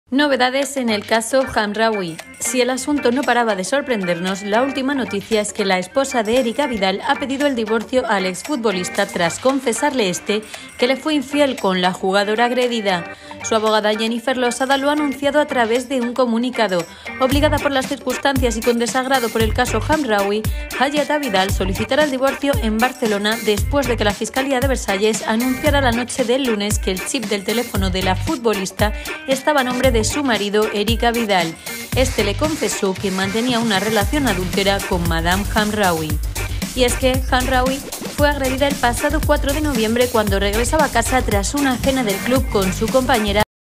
Abidal y Hamraoui mantenían una relación: su mujer le pide el divorcio | Diario AS
La mujer de Eric Abidal le ha pedido el divorcio tras enterarse de que le había sido infiel con la jugadora francesa Hamraoui y las especulaciones de una 'venganza amorosa'.